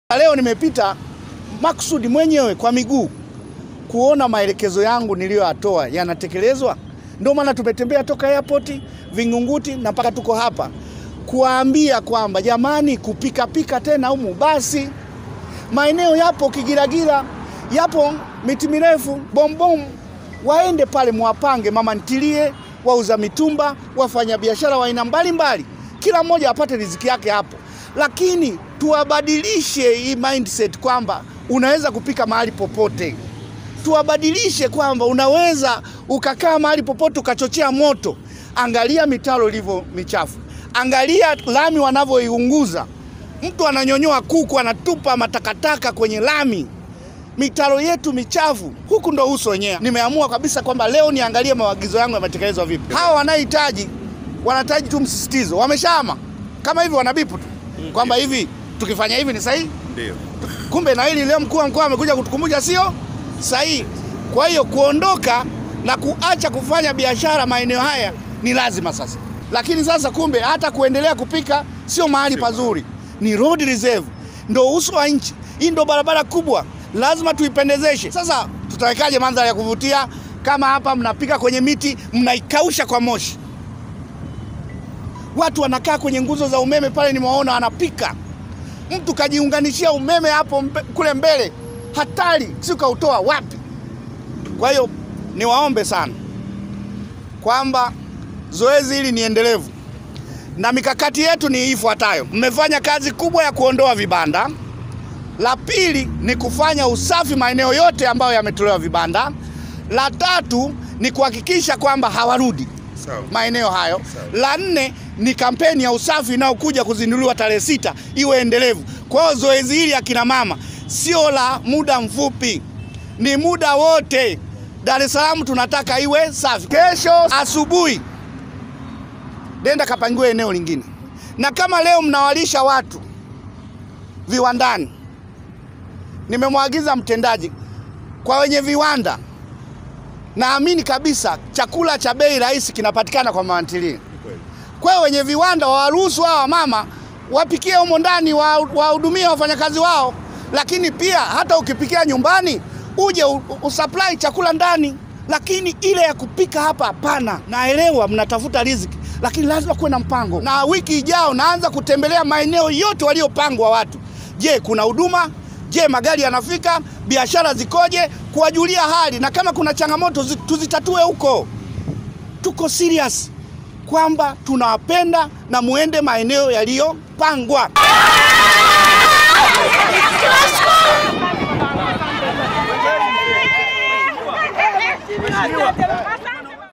RC MAKALLA ATOA ONYO KALI KWA WAMACHINGA WANAOENDELEA KUFANYA BIASHARA PEMBEZONI MWA BARABARA
Mkuu wa Mkoa wa Dar es Salaam Amos Makalla ametoa onyo kwa Wafanyabiashara wanaoendelea kufanya Biashara pembezoni mwa Barabara ya Airport licha ya vibanda vyao kuondolewa kuhama mara moja na kwenda kwenye maeneo yaliyotengwa.

RC Makalla ametoa onyo hilo mapema leo alipotembea mguu kwa mguu kutoka Airport Hadi Chang'ombe kuona utekelezaji wa maelekezo ya Serikali ambapo ameshuhudia baadha ya Wafanyabiashara wakijiiba na kupika vyakula chini ya miti na kuwataka waondoke na kwenda kwenye maeneo yaliyotengwa.

Aidha RC Makalla amesema kuvunja kibanda na kuendelea kupika Chini ni kazi bure hivyo amewaelekeza
Wakurugenzi,Watendaji wa Kata na Mitaa kuhakikisha wanasimamia Wafanyabiashara hao wasirudi upya.

Hata hivyo RC Makalla amesema kwa unyeti wa Barabara ya Airport atahakikisha anasimamia Barabara hiyo iwe na mandhari nzuri.